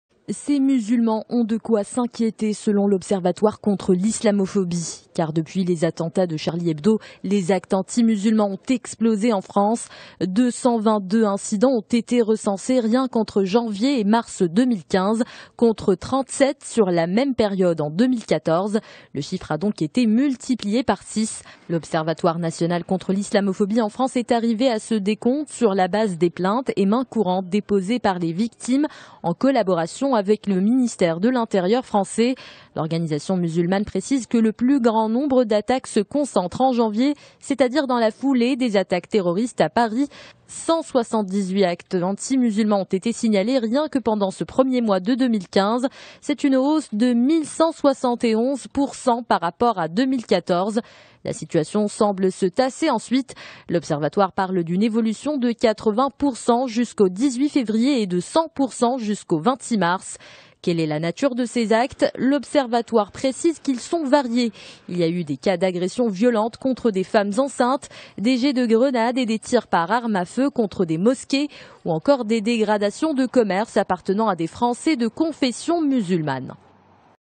France: la montée des actes antimusulmans inquiète la société civile
L'observatoire contre l'islamophobie tire la sonnette d'alarme et publie des chiffres édifiants.

Au premier trimestre de cette année, le nombre d'attaques contre les musulmans a été multiplié par six.

Le récit d'Amina Benkhalek.